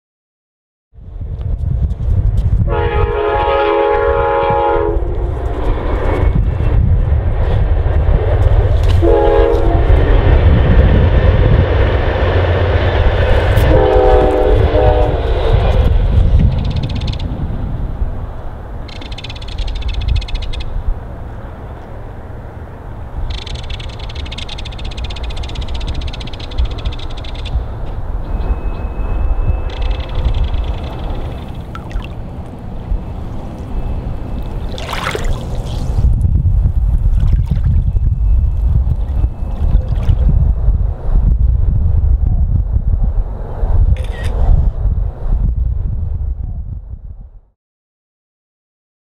Blurred Bird Curb Excerpt
A quick coverage piece of an Abandoned Segregated beach In New Orleans. This part of a multiple coverages taken on a road trip where we stayed with various "Ryan's" In various southern/midwestern states.